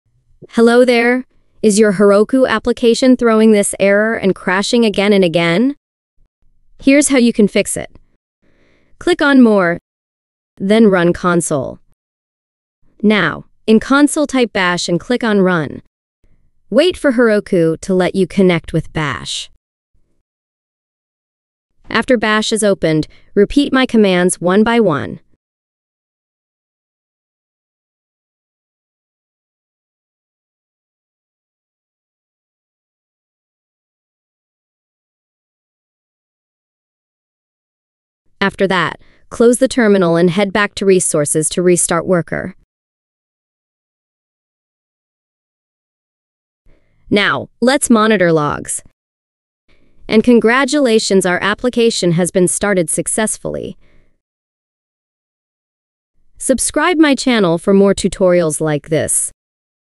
Fix Crash loop in Heroku without Re-deploy
In this video I have shown how to fix JavaScript Heap Out of Memory issue in Heroku without making the app re deployed.

Prerequisite:  Heroku account with eco dyno ( 5$ / Mo ) subscription.